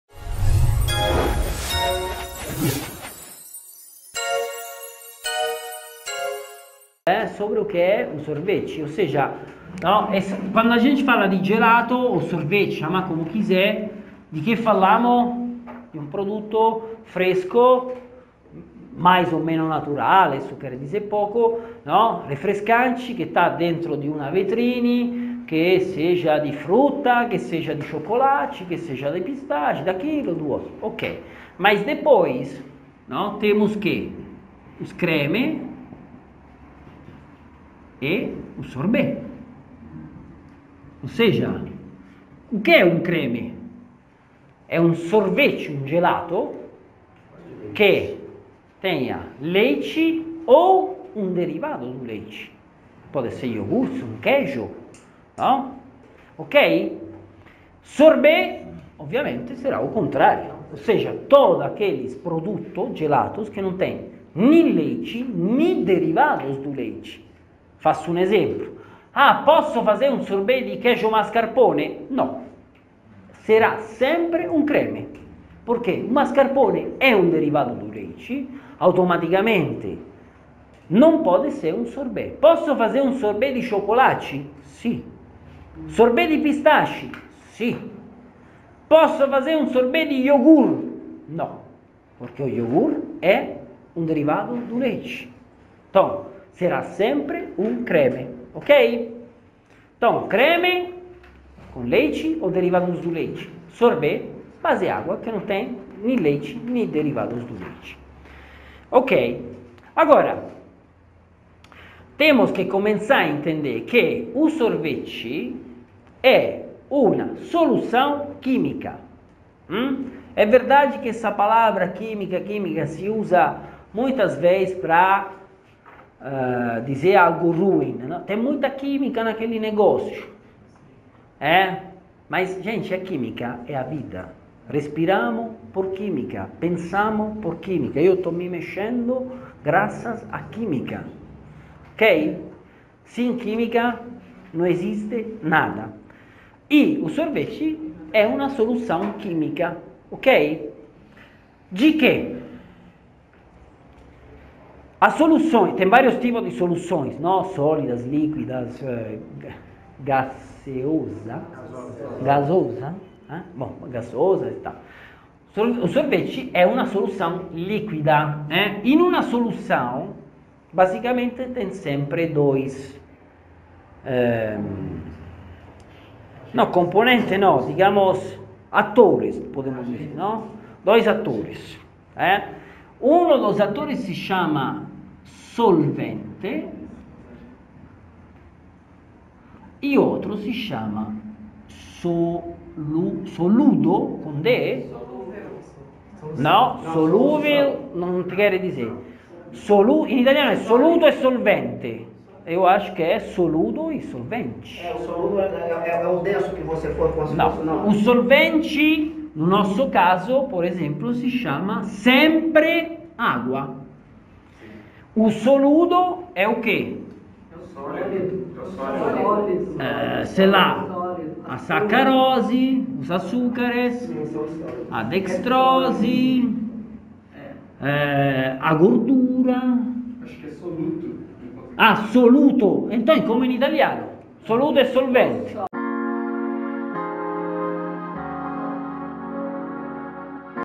Qual a diferença entre Creme e Sorbet ?
Você sabe qual a diferença que determina a diferença entre creme e sorbet?
Então vem com a gente descobrir!

Gostou e quer acompanhar nosso trabalho?